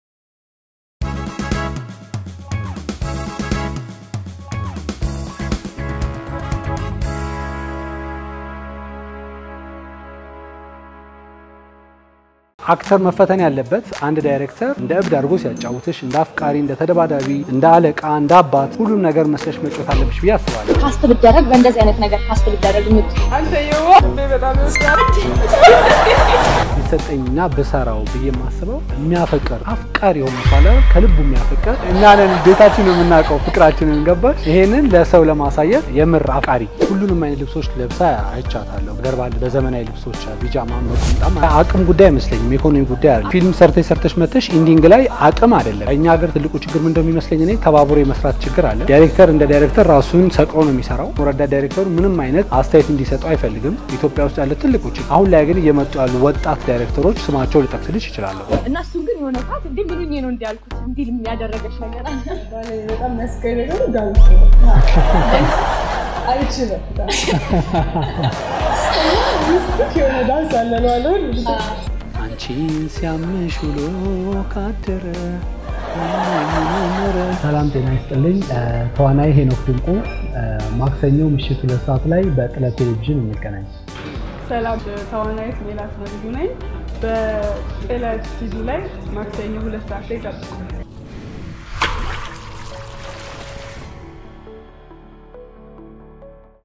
ETHIOPIA አርቲስት ሄኖክ ድንቁ እና ሜላት ነቢዩ ከጥለት ቲቪ ጋር አዝናኝ ቆይታ Henok Dinku Melate Nebeyu ©TILET TV 2020
This program was developed before corona virus infection
CURRENT ISSUES
ENTERTAINMENT
CULTURE, ART & HISTORY
POETICS NEWS
DOCUMENTARY, SHORT MOVIES, MUSIC & SONG